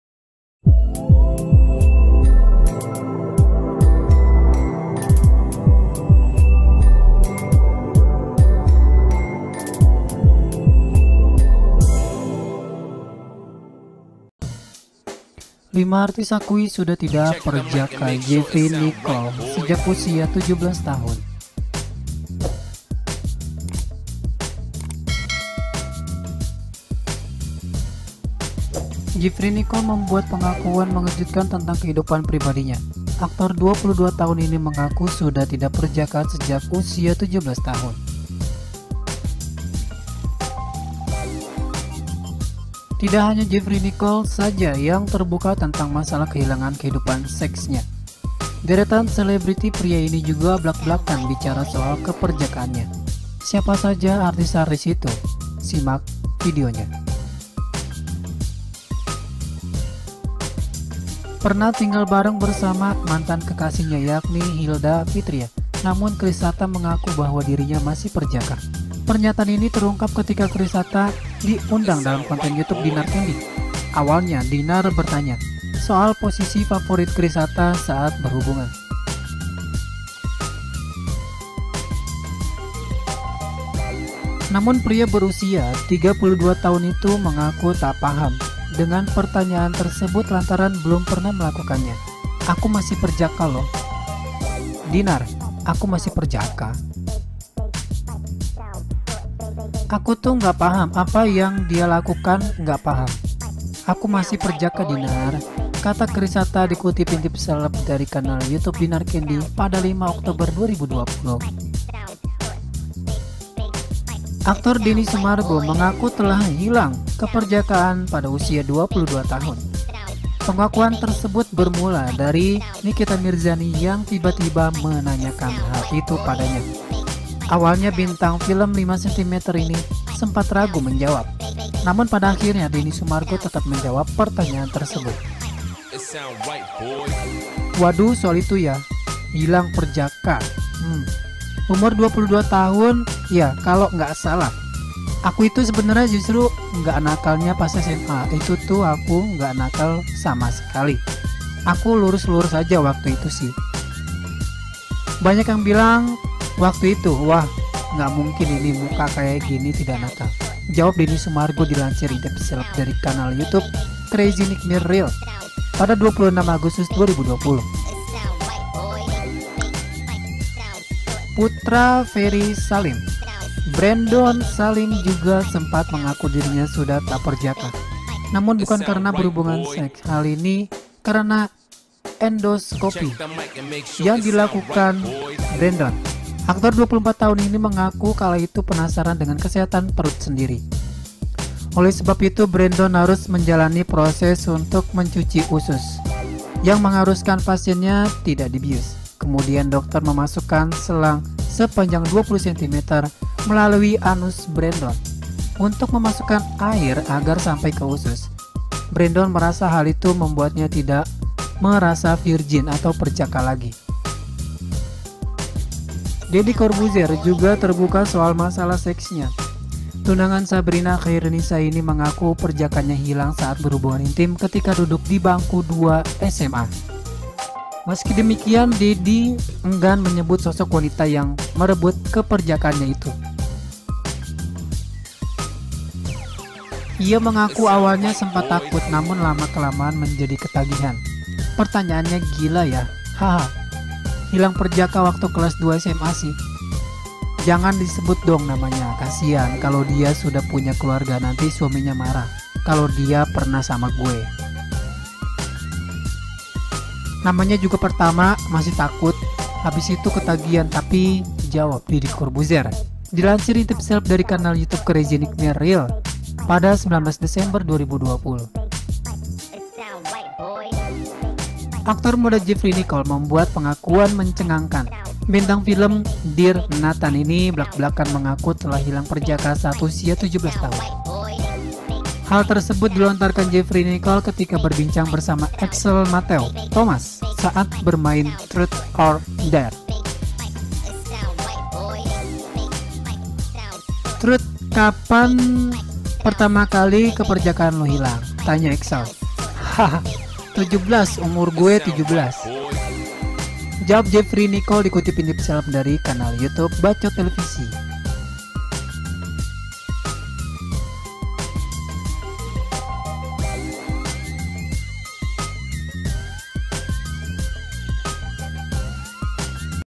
5 Artis ini Akui Sudah Tidak Perjaka Lagi, Ada yg Sejak Usia 17 Tahun
5 Artis Akui Sudah Tidak Pe-rjaka, Jefri Nichol Sejak Usia 17 Tahun -   – Jefri Nichol membuat pengakuan mengejutkan tentang kehidupan pribadinya. Aktor 22 tahun ini mengaku sudah tidak perjaka sejak usia 17 tahun.

Tak hanya Jefri Nichol saja yang terbuka tentang masalah kehilangan kehidupan seksnya. Deretan selebriti pria ini juga blak-blakan bicara soal keperjakaannya. Siapa saja artis-artis itu? Simak artikel berikut ini.